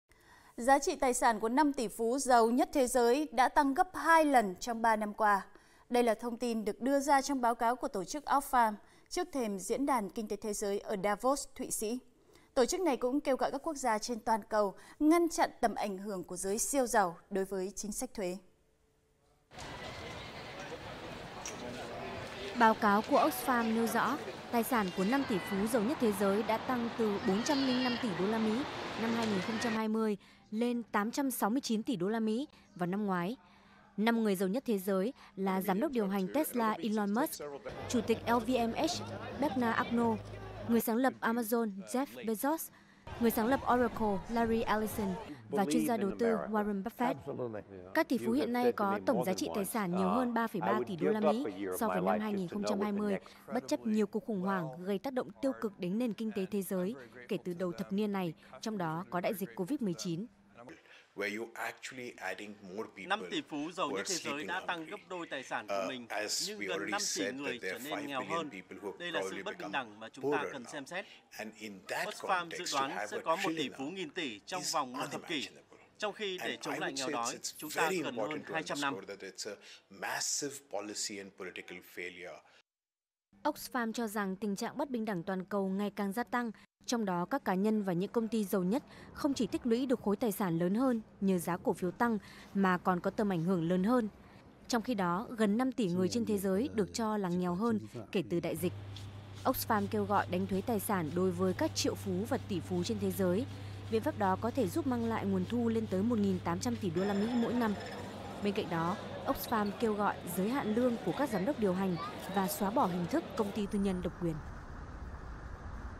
Sắp xuất hiện các siêu tỷ phú nghìn tỷ USD| VTVMoney
Tổ chức chống nghèo đói Oxfam cho biết, thế giới có thể xuất hiện tỷ phú nghìn tỷ USD đầu tiên trong vòng một thập kỷ nữa